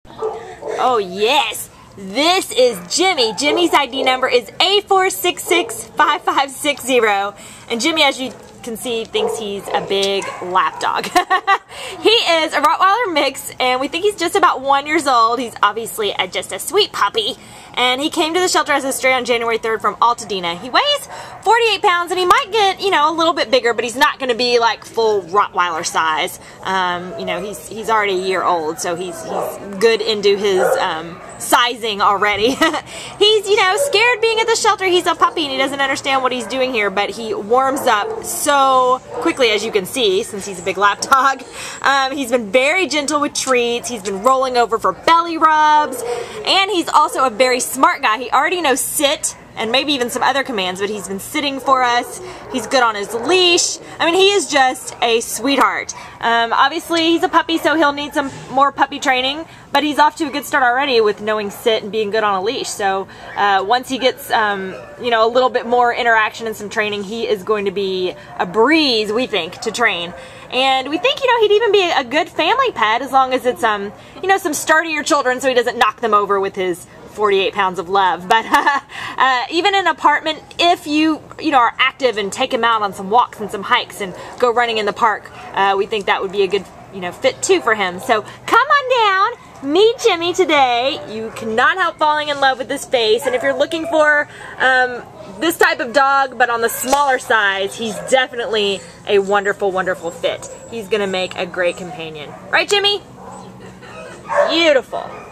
Jimmy A4665560
A4665560 Jimmy is a gentle one-year-old black and tan male Rottweiler mix puppy who was found in Altadena and brought to the Baldwin Park Animal Care Center on January 3rd. Weighing 48 pounds, Jimmy gets along great with other dogs if properly introduced and should take to his puppy training with ease since he is extremely treat-motivated. He is also the king of belly rubs and just a big old lap dog! A little shy what with all the action at the shelter, Jimmy will really shine once he becomes the indoor companion to his forever family.